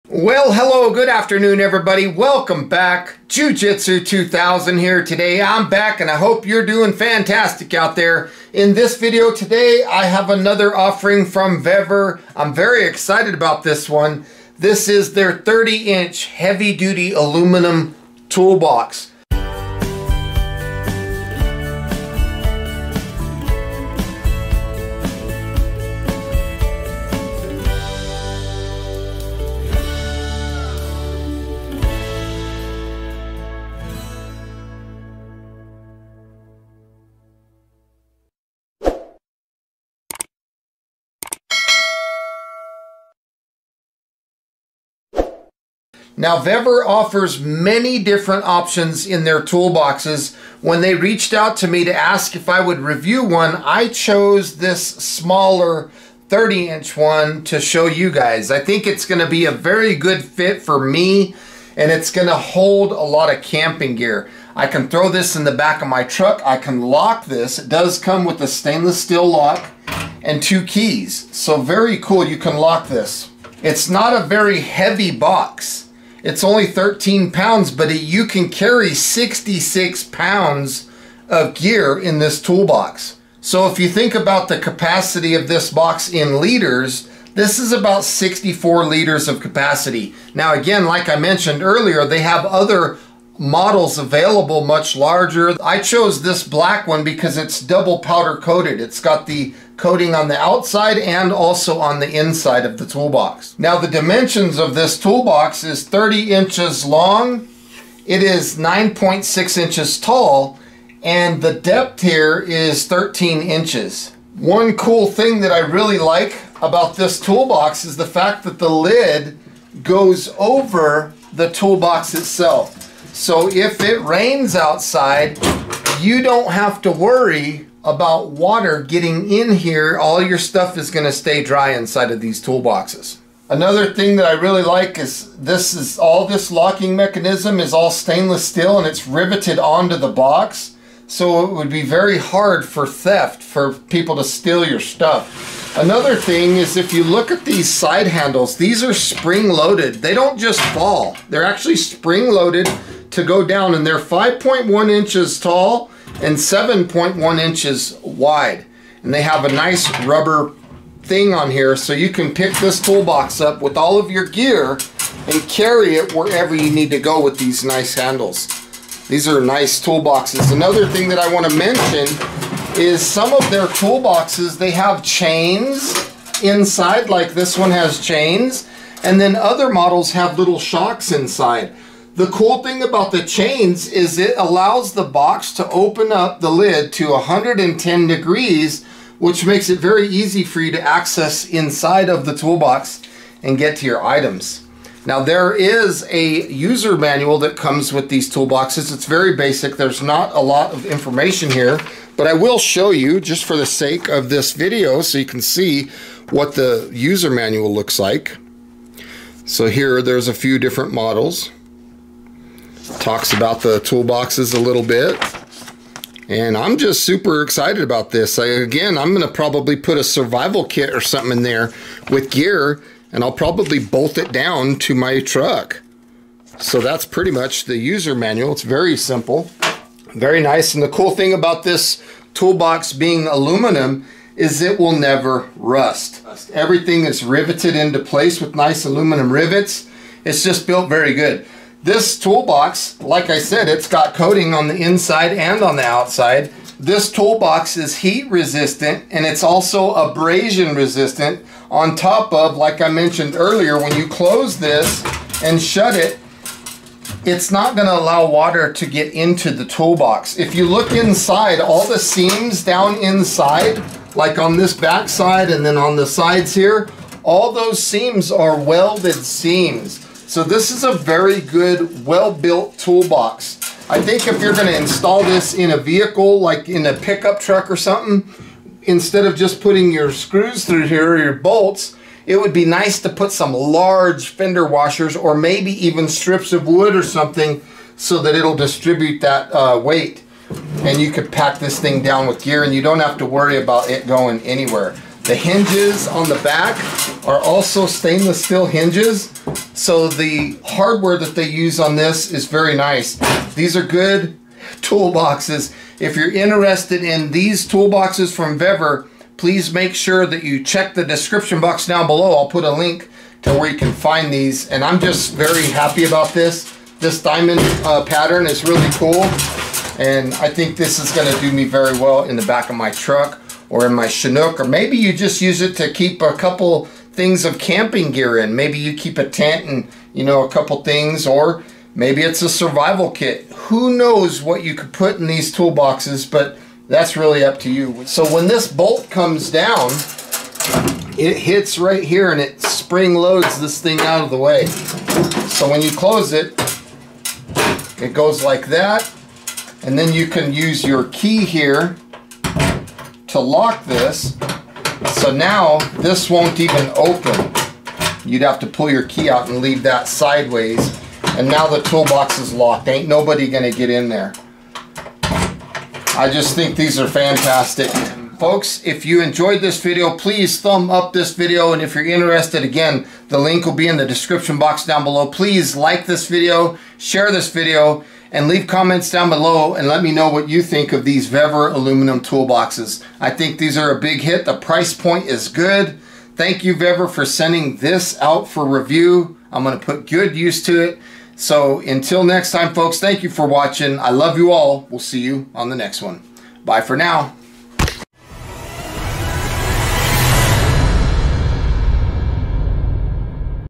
VEVOR Heavy Duty Aluminum Truck Bed Tool Box
In this video I'll look at the Vevor 30 inch heavy duty aluminum tool box. I love it! Enjoy!

    Minimum purchase of $100.00 Applicable: store-wide except on sale products and gift cards